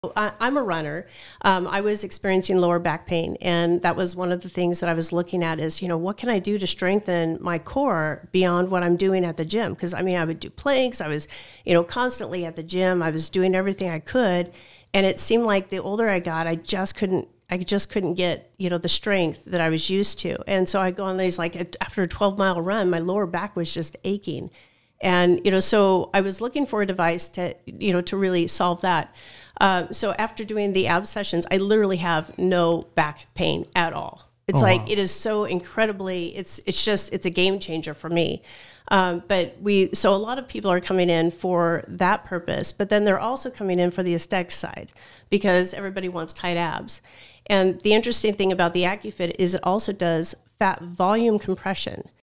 Tight Abs Without the Crunches?
If you’re struggling to build muscle in your core, there are options outside of the gym.